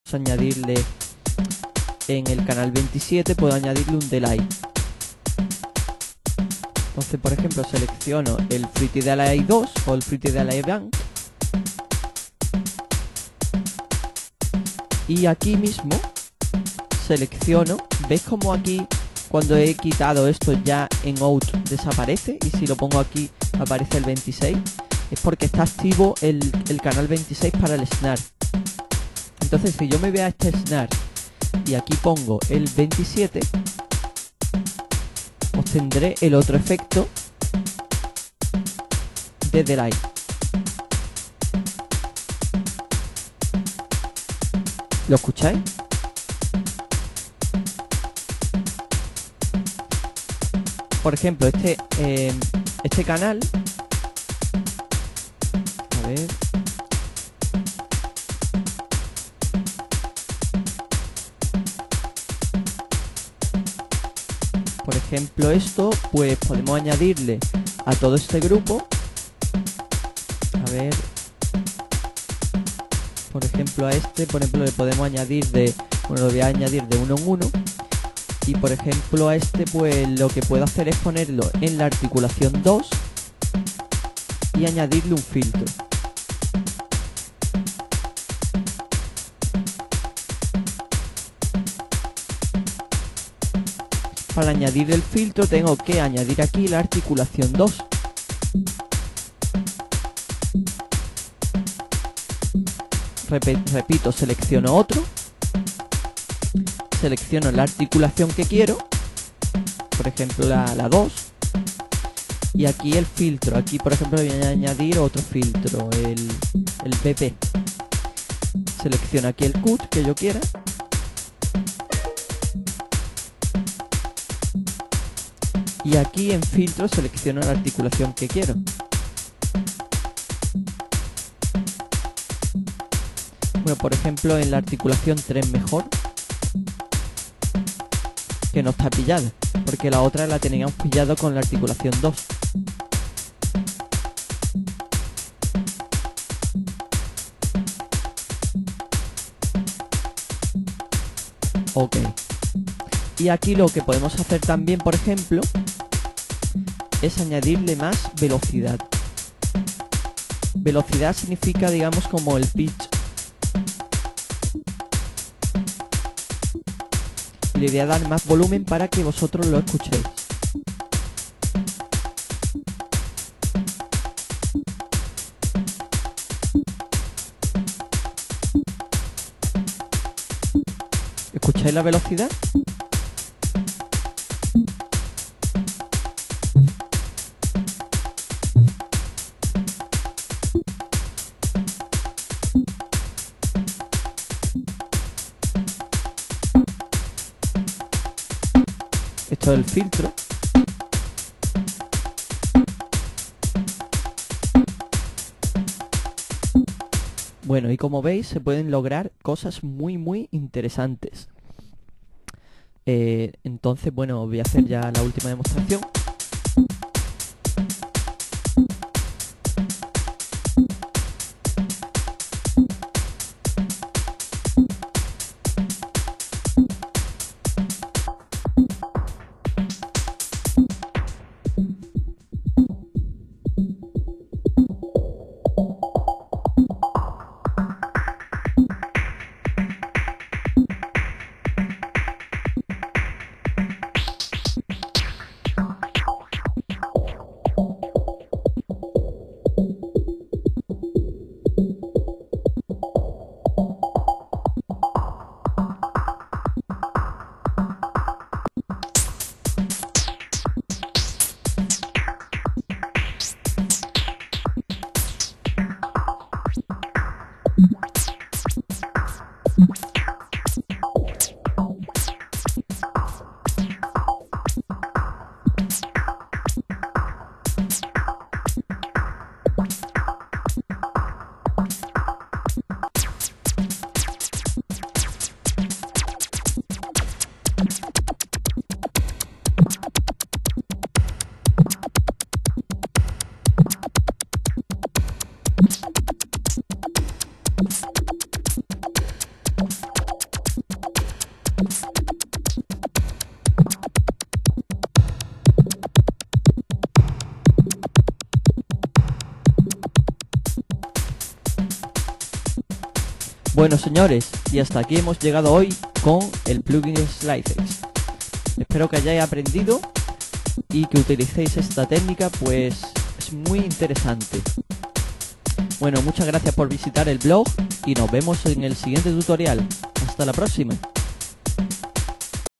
Fl Studio Intermedio 12 (SLYCEX: Troceando Loops) Part 3/3
Este videotutorial especial sobre como trocear loops te dará mucha información útil a la hora de utilirzar el plugin Slycex, y que gracias a este vídeo usted podrá crear loops con más vida, más profesionales y más innovadores.

Muchas gracias